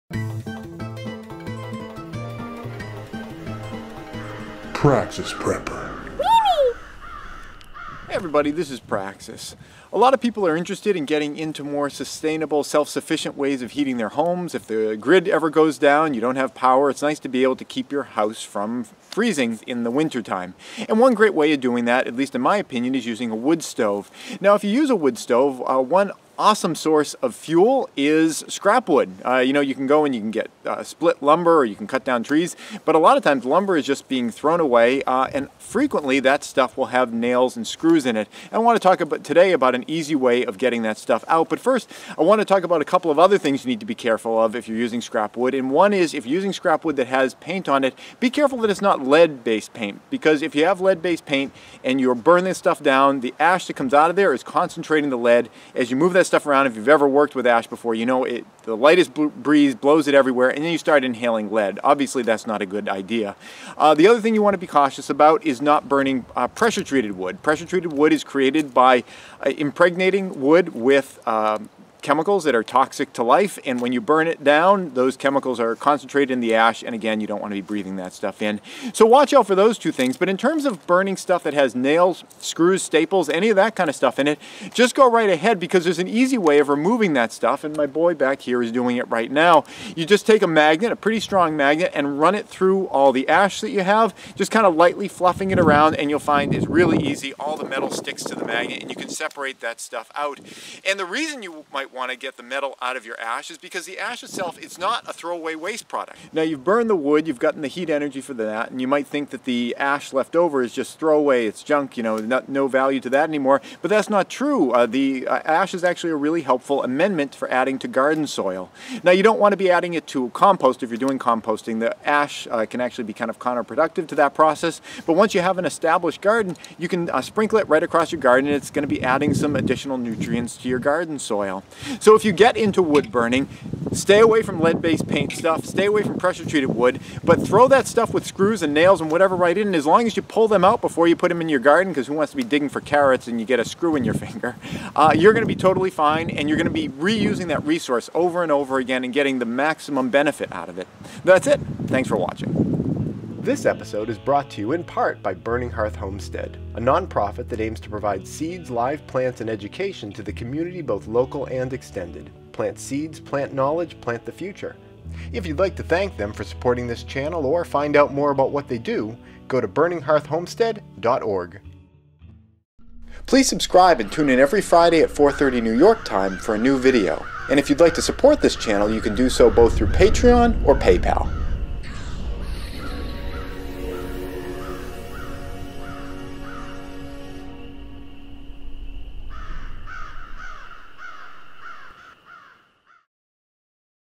Burning Scrap Wood Safely
Can you safely burn wood covered in lead-based paint in your woodstove? How about pressure-treated lumber? And what about wood with nails and screws? Let's talk about it.

Here's an awesome magnet that's a lot better than the one I am using in this video